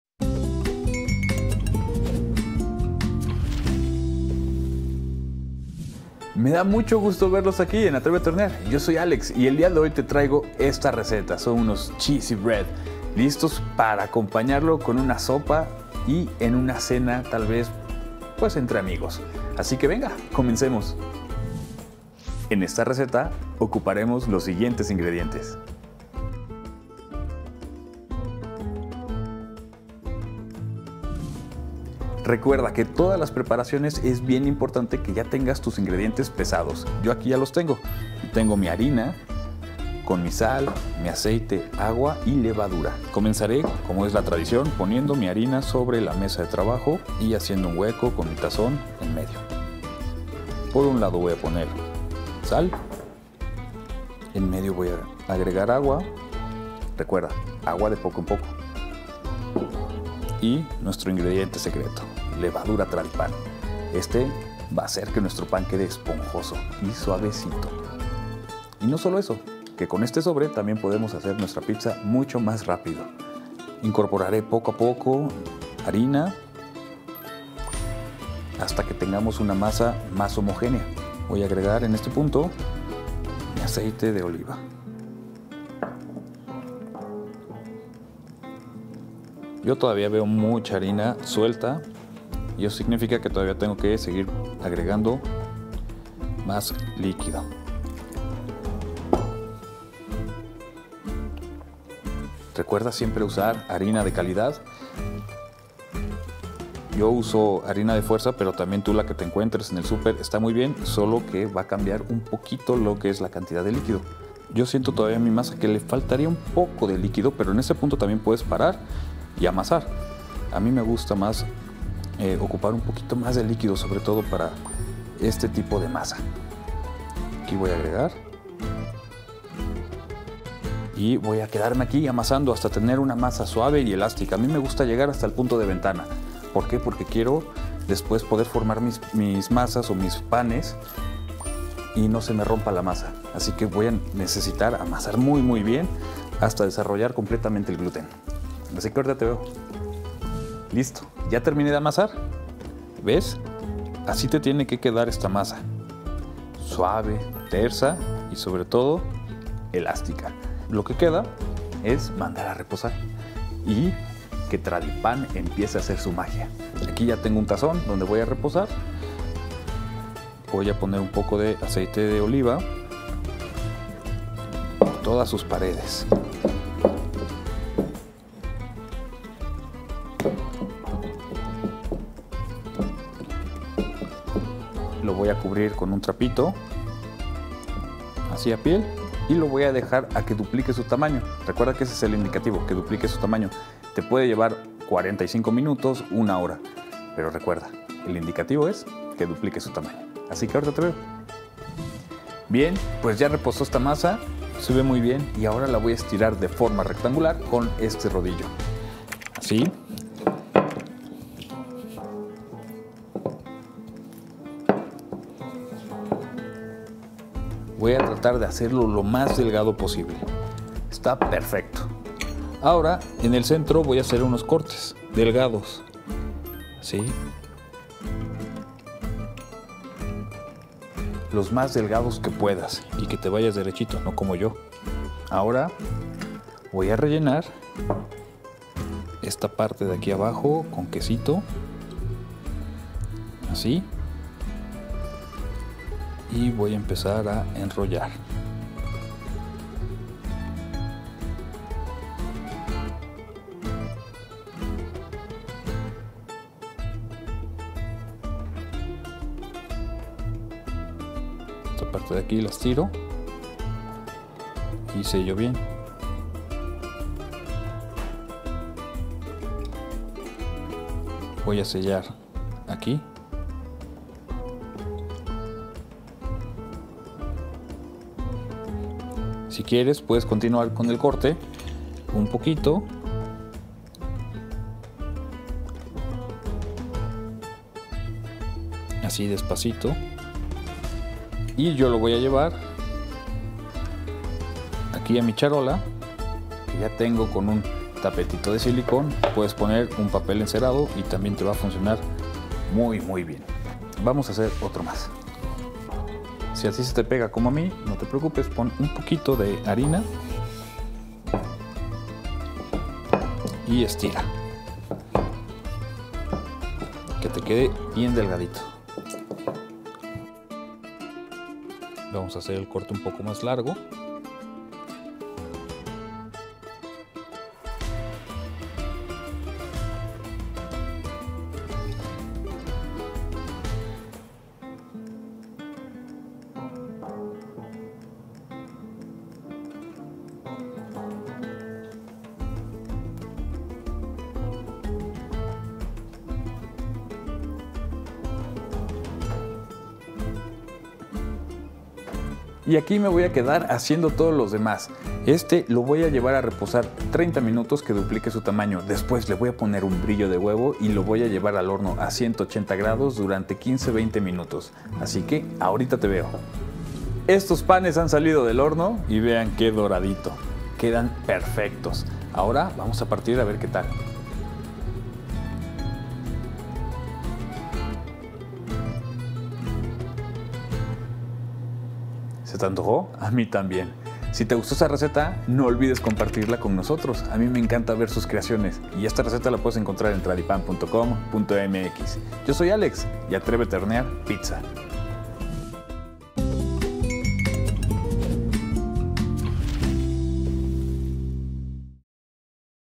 ¡Deliciosos Cheese Breads! Prepáralos con Tradipan
🧀🍞 Perfectos para cualquier momento, ¡estos cheesebreads son irresistibles!
 Alex nos muestra cómo prepararlos.

INGREDIENTES
- 500 gramos de harina de trigo
- 12 gramos de sal
- 1 sobre de levadura Tradipan®
- 325 mililitros de agua
- 40 mililitros de aceite de oliva
- Queso mozzarella

¡Comenta este vídeo si deseas prepararlos! 💛